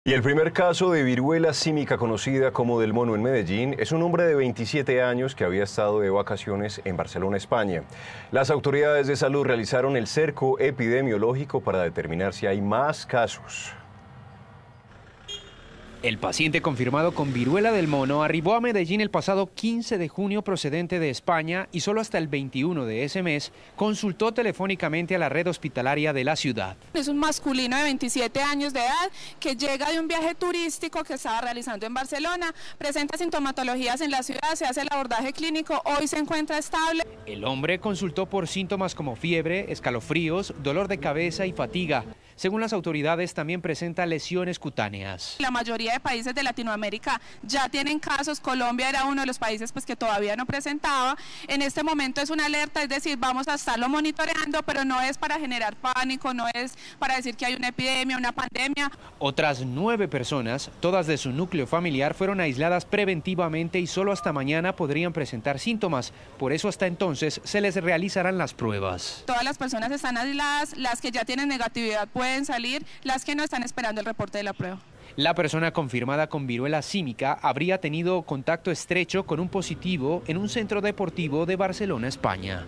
Nueve familiares de joven en Medellín que tiene viruela del mono fueron aislados preventivamente
El primer caso de viruela del mono en Medellín es un joven de 27 años que había estado de vacaciones en Barcelona, España, y arribó al país el pasado 15 de junio. Nueve de sus familiares fueron aislados preventivamente y aguardan a la posible aparición de síntomas para realizarles pruebas.

“Todas las personas están aisladas, las que ya tienen negatividad pueden salir, las que no están esperando el reporte de la prueba”, recalcó Andree Uribe, secretaria de Salud de la ciudad.